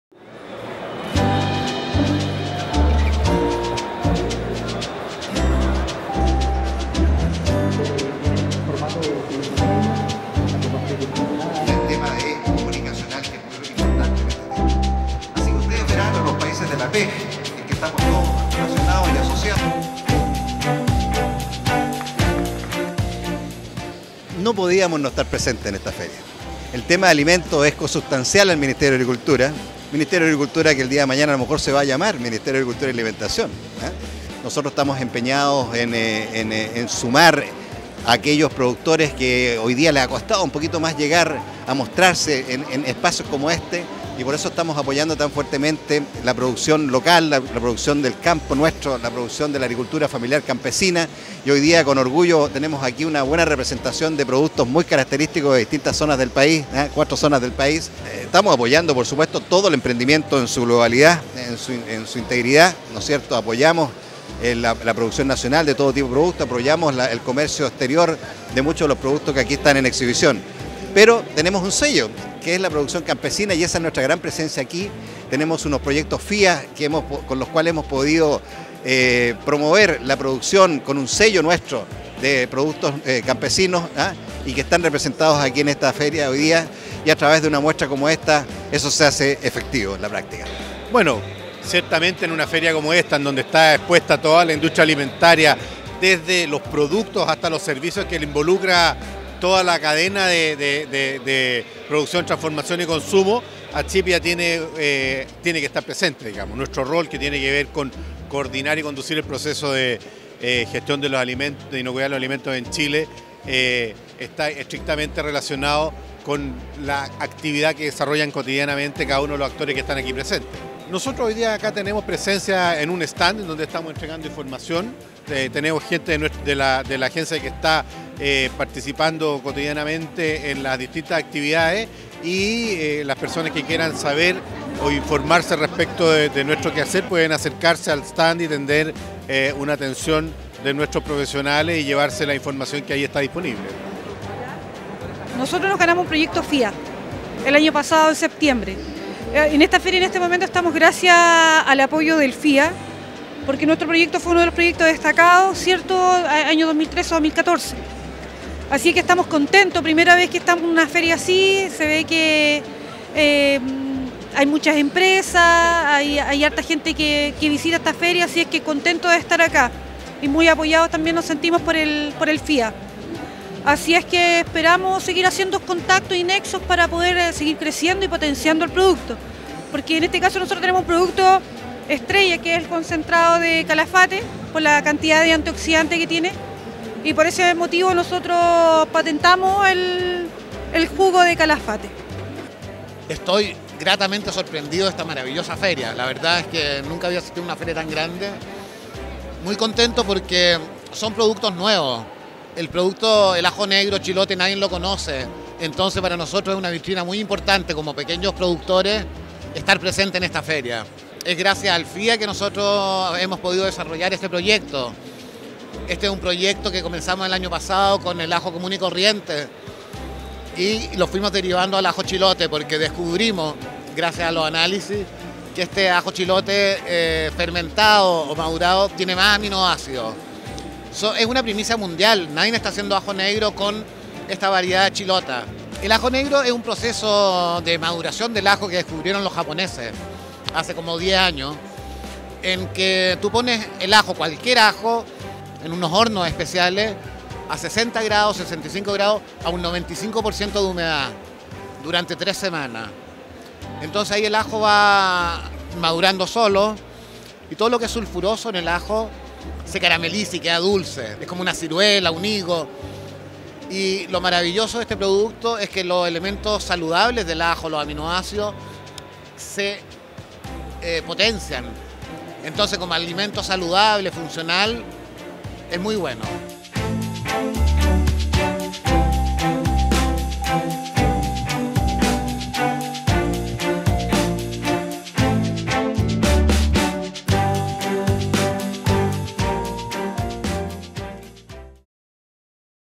Inauguración de Espacio Food & Service 2014
Ministro (s) de Agricultura, Claudio Ternicier, participó de la ceremonia inaugural de Espacio Food & Service 2014 en Espacio Riesco.